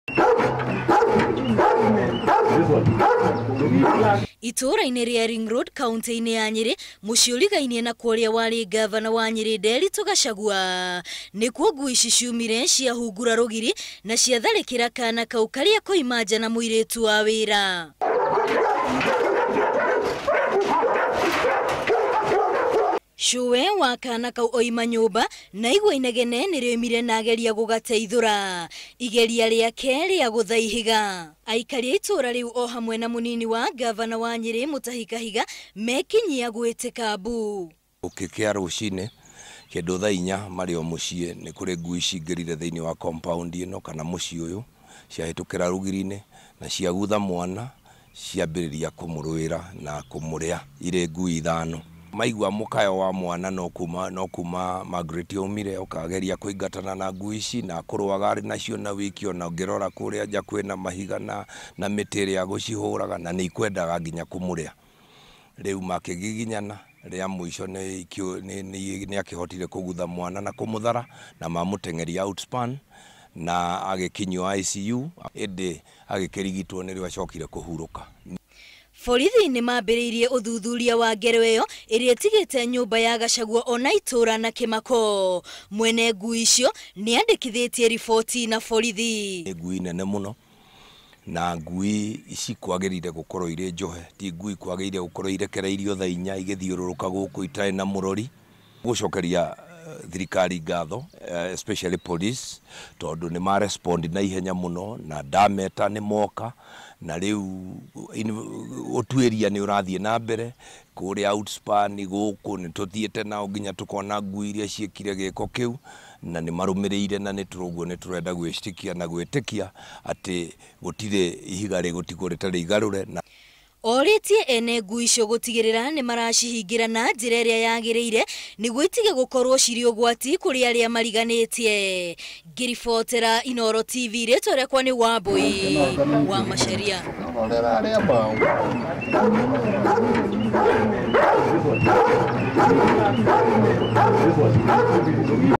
Kaguka ka mutigairi Ngabana Nderitu Gachagua kuuragwo ni ngui
Family ya  uria waari ngabana wa Nyeri Nderitu Gachagua  niiracakaya gikuu kia mwana  wa miaka iiri na nuthu uria urokire gutharikiirwo ni ngui cia mucii mariganitie.
Mwana ucio ni wa muriu wa ngabana Gachagua.